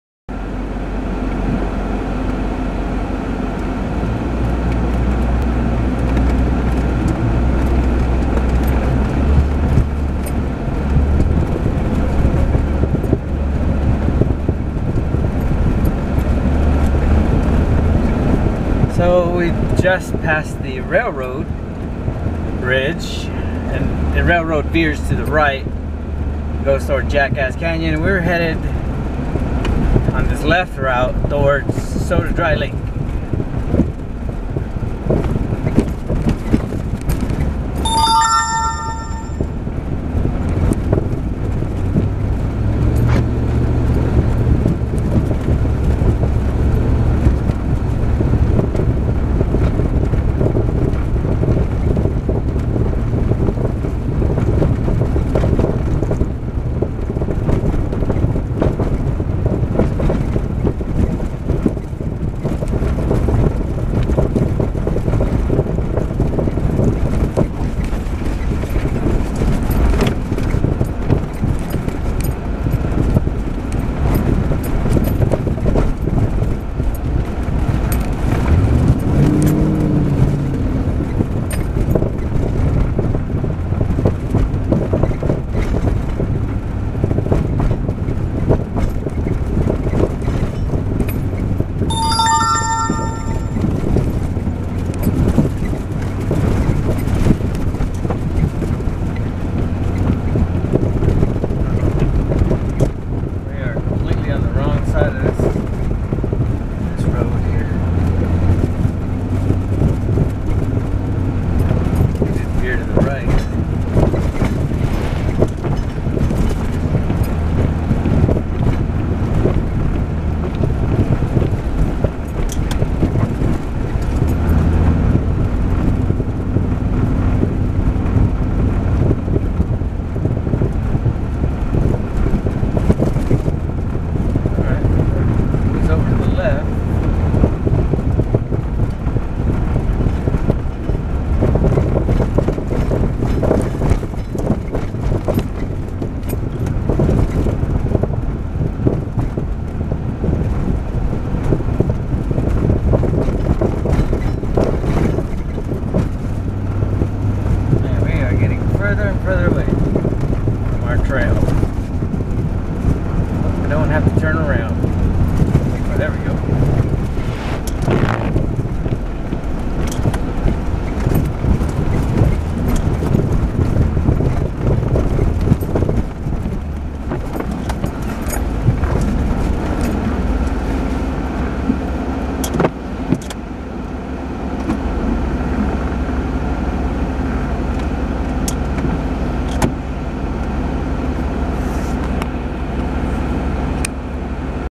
Splish Splashing on Mojave Road
Leaving the Train Bridge, Northbound, on Mojave Road, there were lots of opportunities to splash in the water.
Mojave Road was built following water routes and water holes when travel by foot, horse and camel were the only means of transportation, and in the desert, this could prove very important.